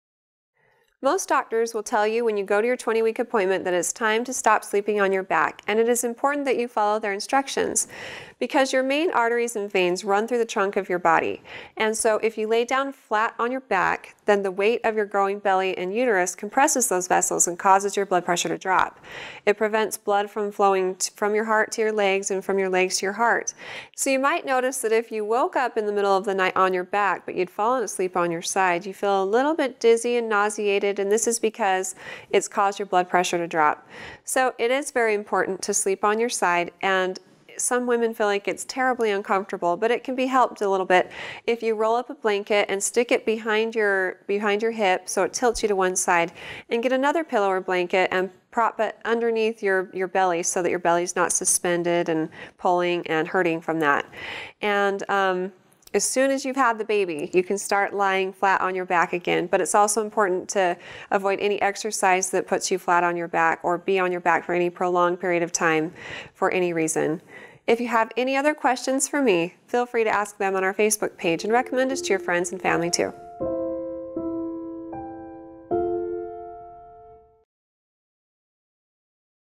Does it really matter if you sleep on your back when pregnant?
Most doctors will tell you that when you go to your 20 week appointment that it's time to stop sleeping on your back. And it is important that you follow their instructions, because your main arteries and veins run through the trunk of your body. And so if you lay down flat on your back, then the weight of your growing belly and uterus compresses those vessels and causes your blood pressure to drop. It prevents blood from flowing from your heart to your legs, and from your legs to your heart. So you might notice that if you woke up in the middle of the night on your back, but you'd fallen asleep on your side, you feel a little bit dizzy and nauseated, and this is because it's caused your blood pressure to drop. So it is very important to sleep on your side.

And some women feel like it's terribly uncomfortable, but it can be helped a little bit if you roll up a blanket and stick it behind your hip so it tilts you to one side, and get another pillow or blanket and prop it underneath your belly so that your belly is not suspended, and pulling, and hurting from that. And as soon as you've had the baby, you can start lying flat on your back again. But it's also important to avoid any exercise that puts you flat on your back, or be on your back for any prolonged period of time for any reason.